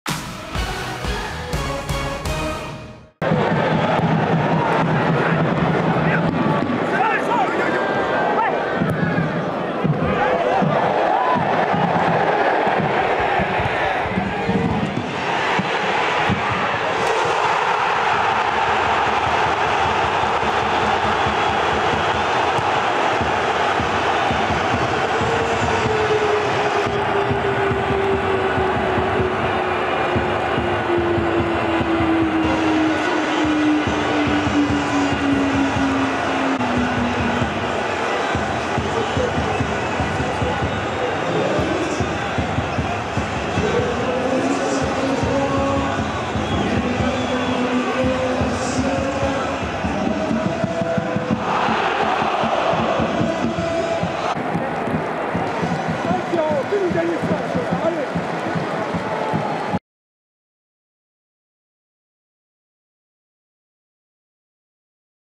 But Mohamed BAYO (65' - CF63) CLERMONT FOOT 63 - RC LENS (2-2) 21/22
"CLERMONT FOOT 63 - RC LENS (2-2) But de Mohamed BAYO (65' - CF63)  + Tous les buts de CLERMONT FOOT 63 - RC LENS en vidéo.

Ligue 1 Uber Eats - Saison 2021/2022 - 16ème journée STADE GABRIEL MONTPIED - mercredi 1 décembre 2021

Buteurs : Johan GASTIEN (12' csc - RCL) / Yohann MAGNIN (41' - CF63) / Seko FOFANA (47' - RCL) / Mohamed BAYO (65' - CF63)